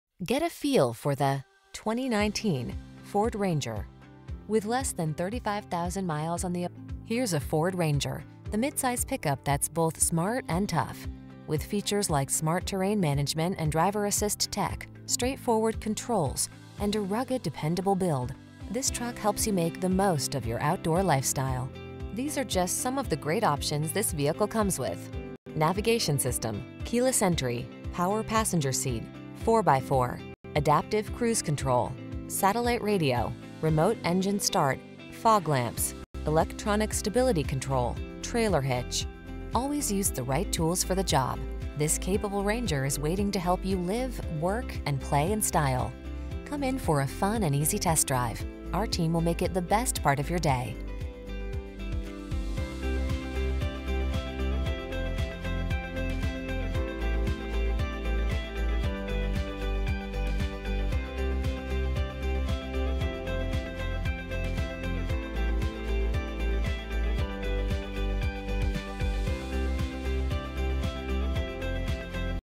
2019 Ford Ranger Staten Island, Jersey City, Bayonne, Woodbridge, Old Bridge, NY A60985A
Ingot Silver Used 2019 Ford Ranger Lariat  available in Staten Island, New York at Dana Ford. Servicing the Jersey City, Bayonne, Woodbridge, Old Bridge, New York area.

Dana Ford
266 West Service Rd